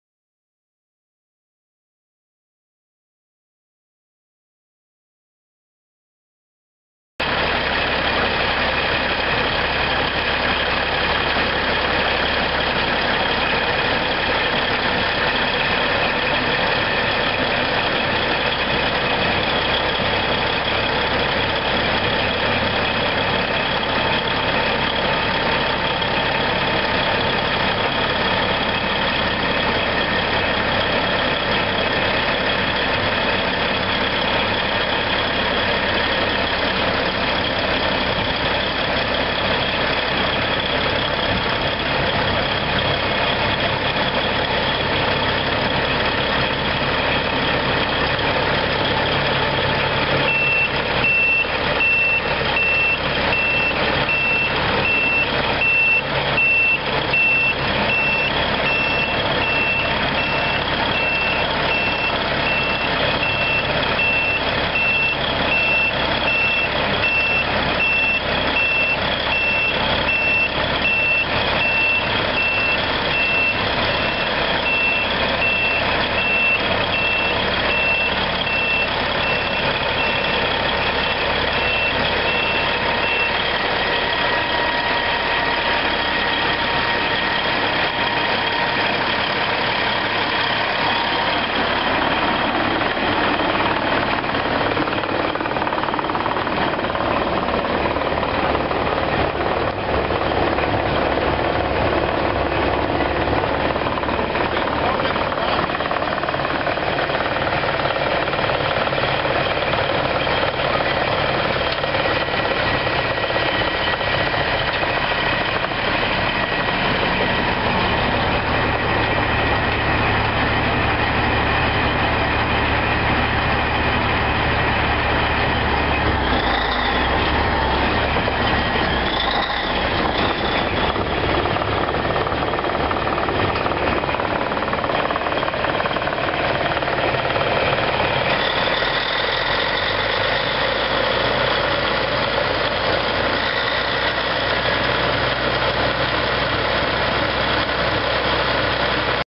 1991 Caterpillar RR250 reclaimer for sale | sold at auction August 13, 2015
1991 Caterpillar RR250 reclaimer
5,645 hours on meter
Open station
Suspension seat
Vandal guard
Two post roll-over protection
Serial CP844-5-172
ROPS test weight 42,000 lbs
8&#039;W drum
Water system
Gorman Rupp pump
Duramax 23.5-25L-2 front tires
15.5-25 rear tires
37,010 lbs. base operating weight
Manual
One owner
Drum housing has been replated/reinforced
Serial 6ED00190

It was located in Mcpherson, KS, and we sold it to the highest bidder for $40,150.